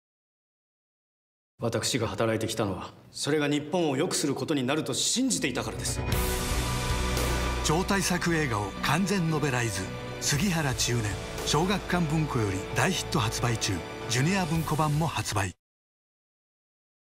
杉原千畝（小学館文庫）ＴＶＣＭ
あなたは知っていますか？第二次世界大戦下、ユダヤ難民に日本通過ヴィザを発給し6000人もの命を救った一人の日本人がいたことを。その男の名は、杉原千畝。感動の超大作映画を完全ノベライズ！『杉原千畝』小学館文庫より絶賛発売中。詳しくはこちら